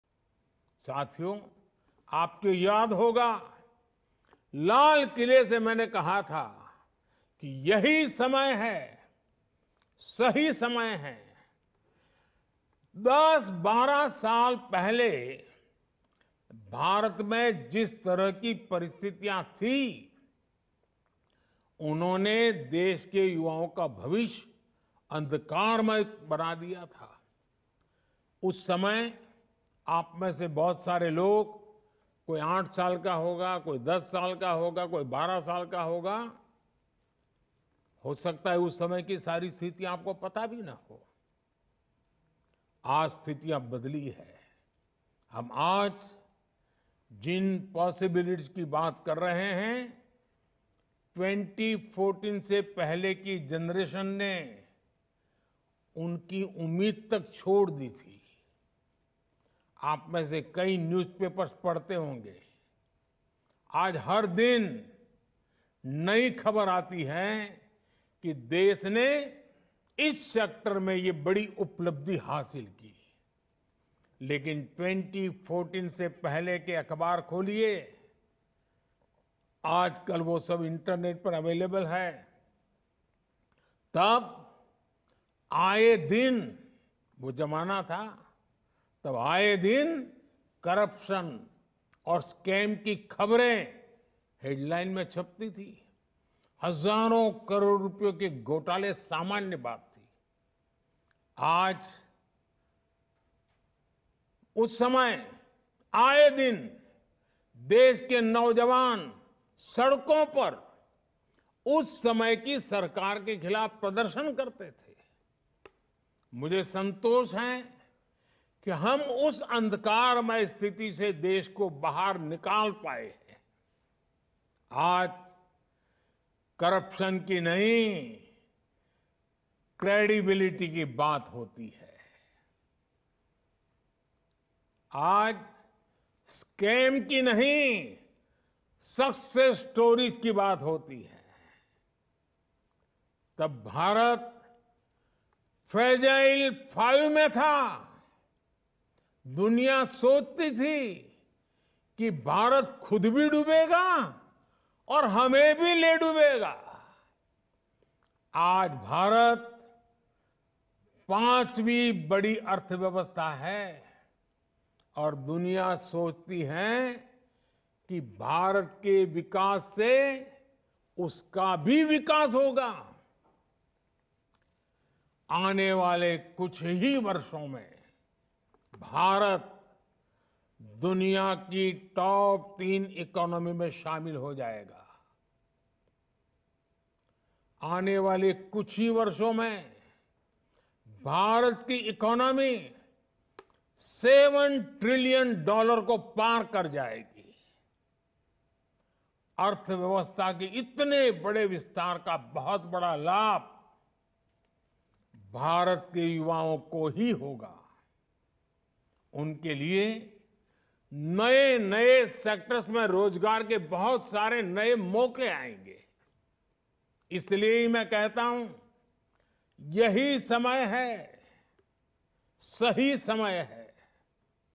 Over last 10 years, the Govt has transformed from corruption to credibility in governance: PM Modi
Addressing Nav Matdata Sammelan, Prime Minister Narendra Modi said that over last 10 years, the Government has transformed from corruption to credibility in governance and from scams to success stories in achievements.” He added that we have only facilitated opportunities for India across sectors. “In the future, India will become the 3rd largest economy with a GDP of more than $7 trillion.”